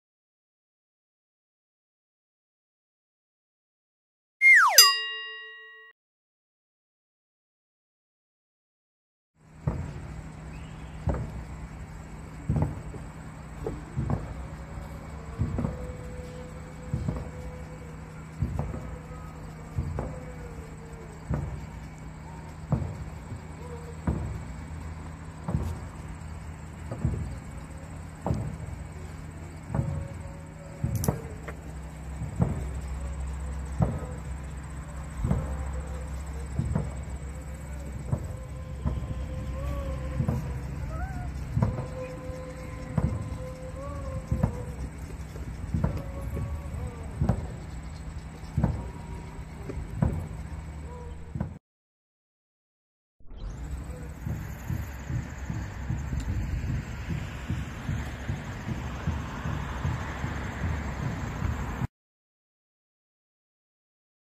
TOTEM POLE at GARDENS AT THE LEAF IN ASSINIBOINE PARK WINNIPEG MANITOBA
A  new 21 metres tall totem pole raised in Gardens at The Leaf at Assiniboine Park @FriendlyPrairie